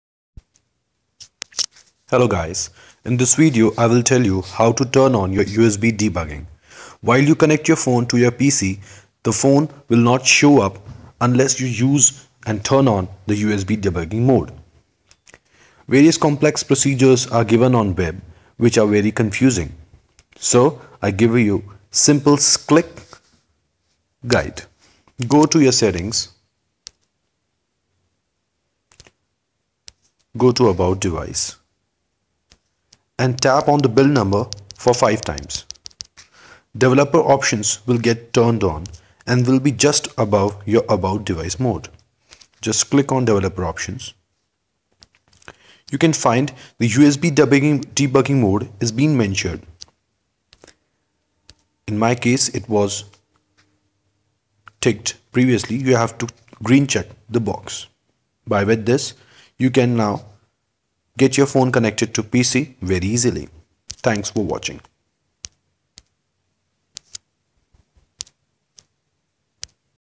How to enable Usb Debugging? Solution
This guide helps to enable developer options and enabling usb debugging. Kindly like this video. If you find this helpful.